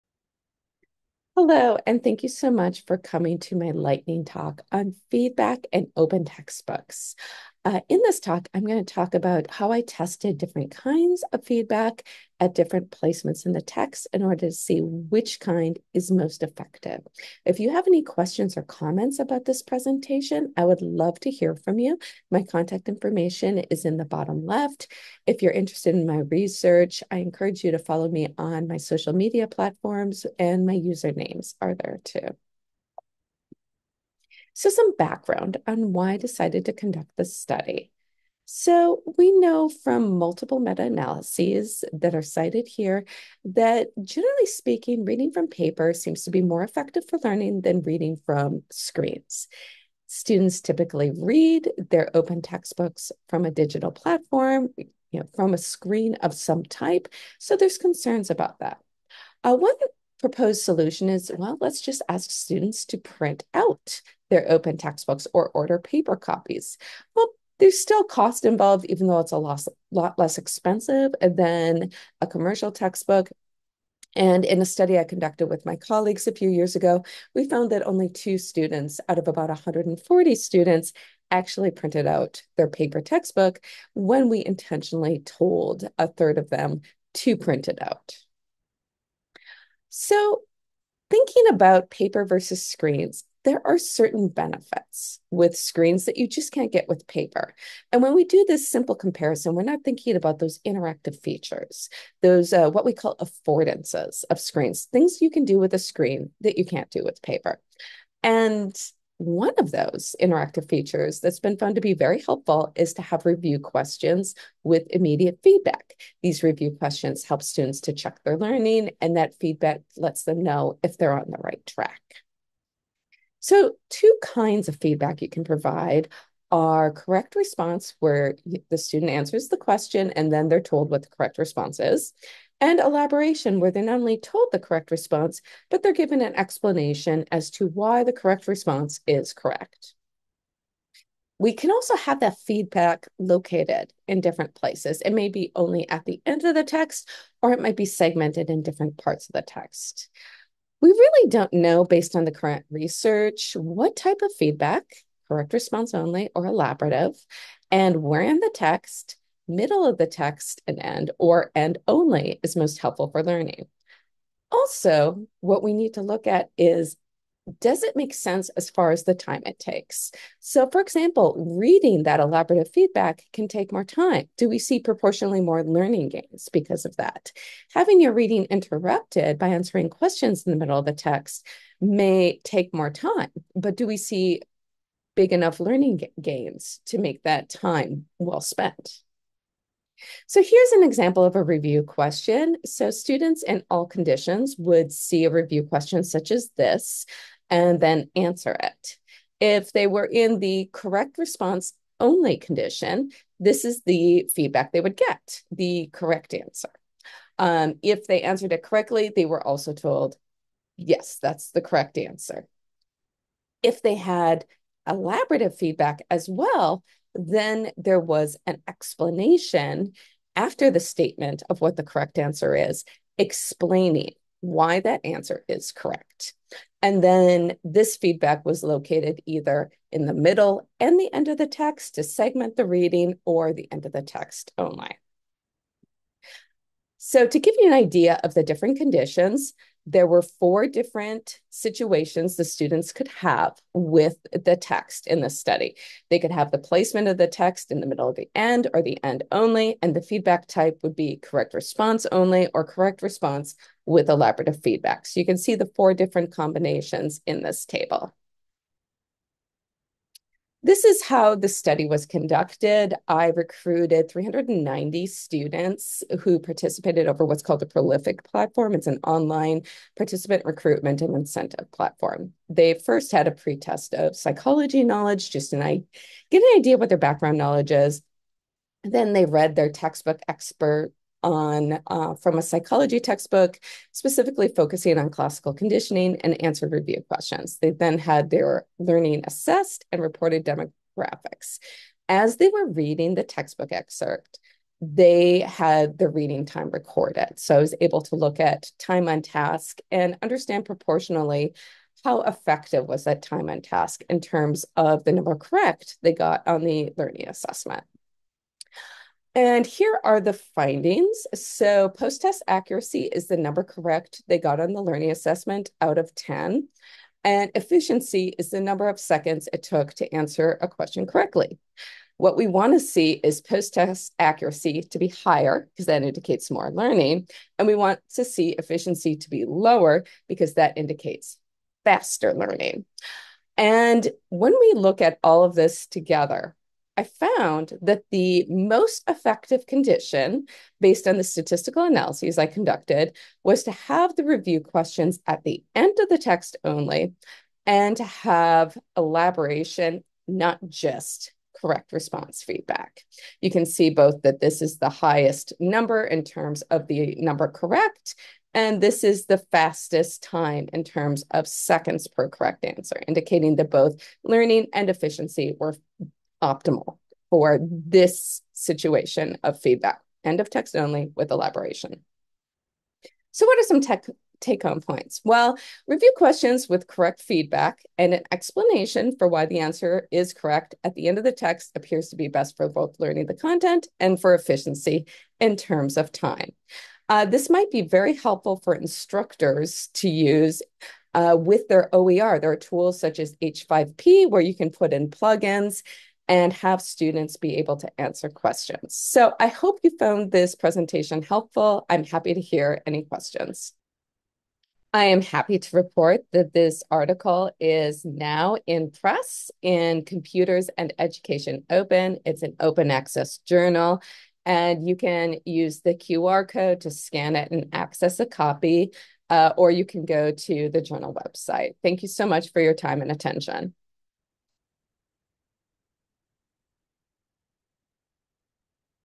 Feedback in open textbooks: what kind and where is best
This is a presentation for opened 2024 about an experiment I conducted testing how different kinds of feedback while reading affected learning the content.
Clinton-Lisell, V. (2024). Feedback for learning from text: What kind and where in the text is most effective? Computers and Education Open, 7, 100216.